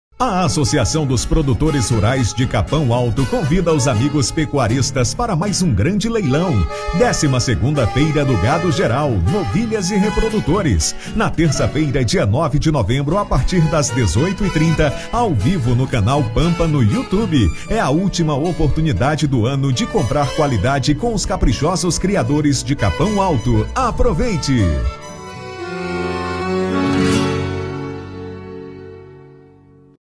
Vem aí, Feira de Gado Geral e Reprodutores em Capão Alto-SC!
Dia 09 de Novembro (Terça-Feira)

🕒 Transmissão ao vivo às 18:30​ horas.